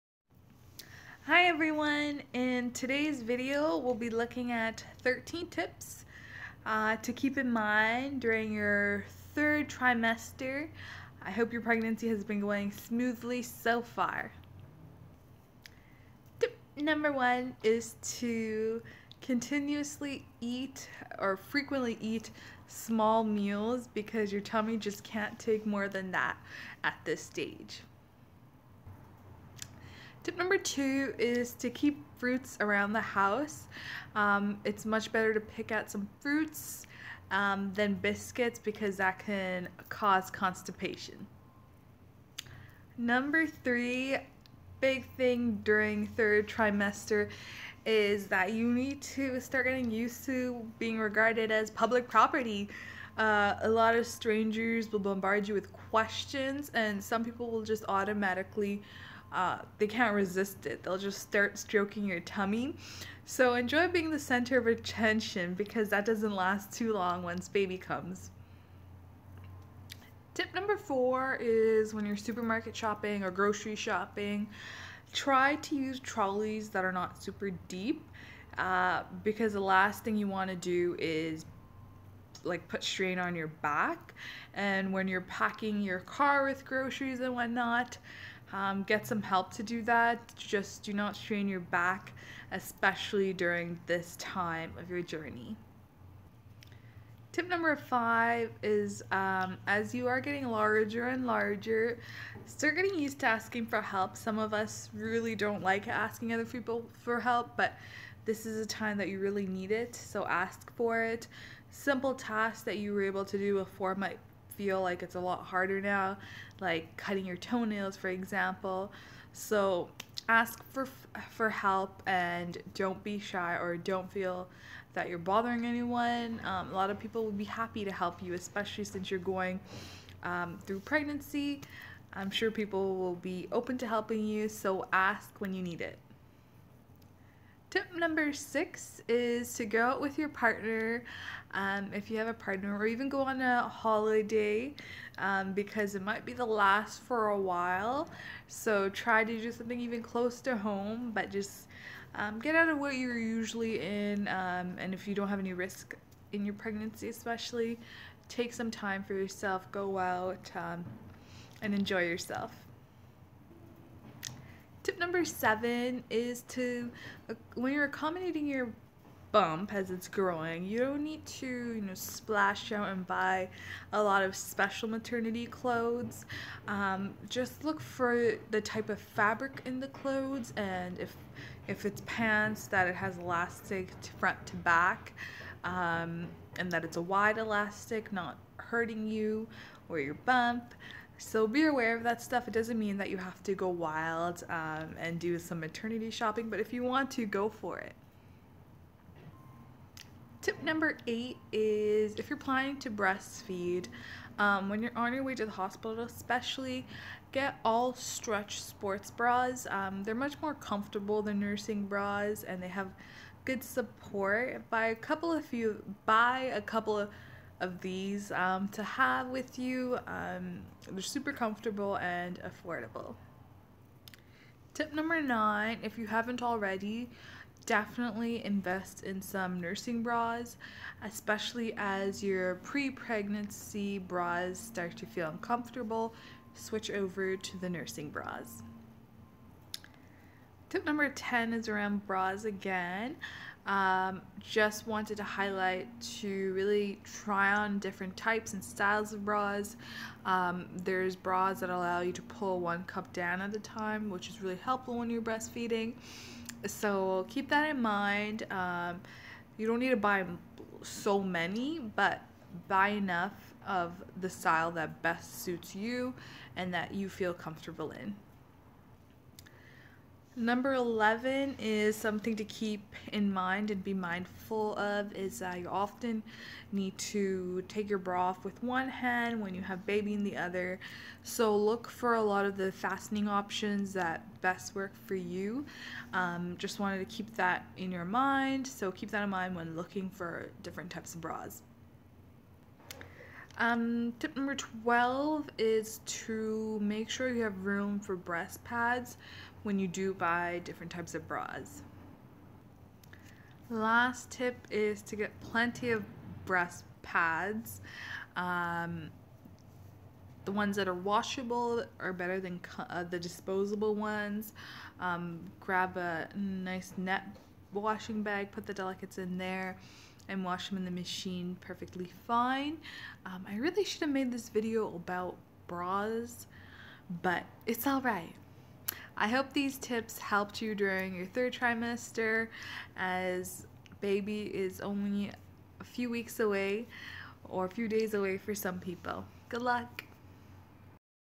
13 Tips: 3rd Trimester Pregnancy
Welcome to your third trimester! Over the coming weeks, your baby will grow from about the size of a plate of pancakes up to a full-fledged turkey (if baby decides not to come out on time, that is!). And you’ll go through a lot of changes, too.

Have a request on what we should post next? If you're still reading this, comment with ♥

STAY HAPPY